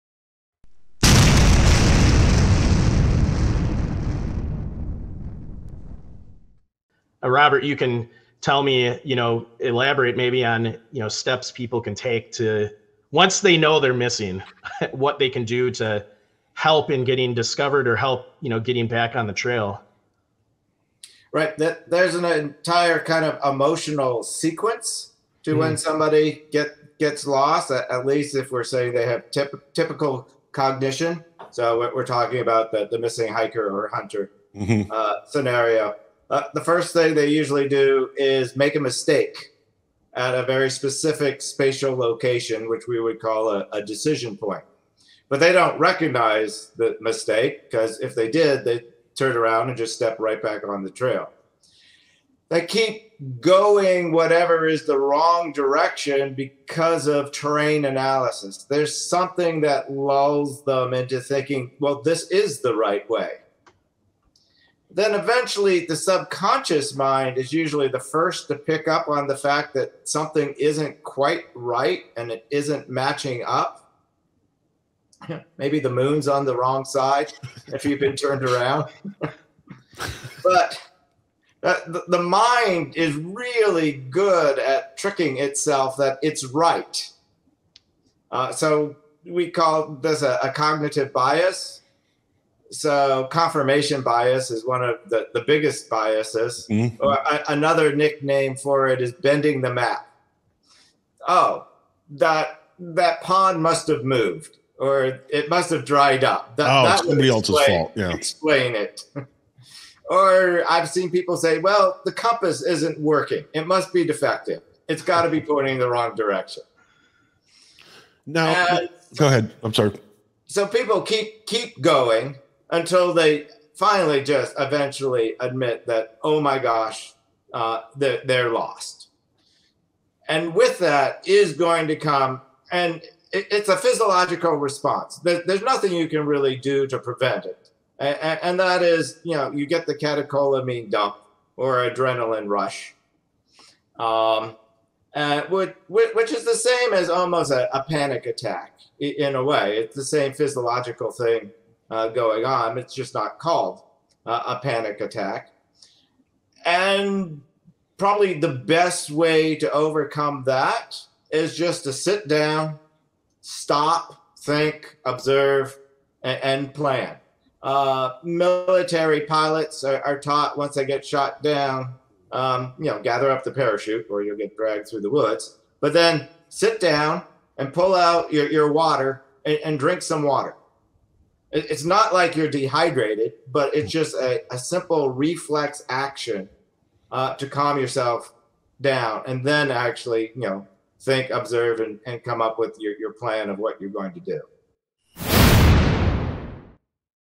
LOST PERSON BEHAVIOR
Search and Rescue Expert ROBERT J KOESTER discusses LOST PERSON BEHAVIOR, and what goes through he mind of EVERYONE when they are lost in the wilderness.